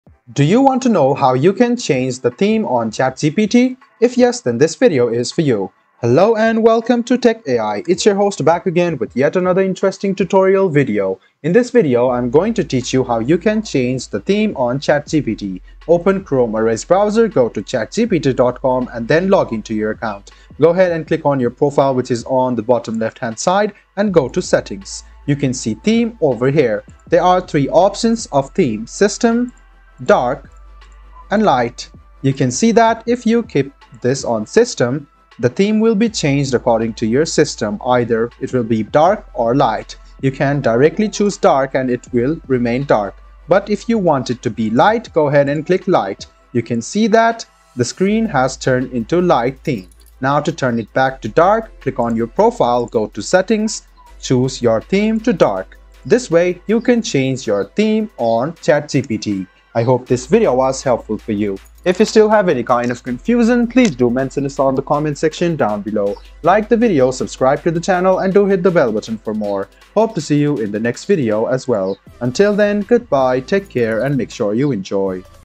How to Change Theme on ChatGPT 2024 | Edit Theme on ChatGPT
Watch this tutorial to learn how to change the theme on ChatGPT. Whether you prefer a light or dark mode, we’ll guide you through the steps to switch themes and personalize your interface. Follow along to update your ChatGPT theme and improve your user experience.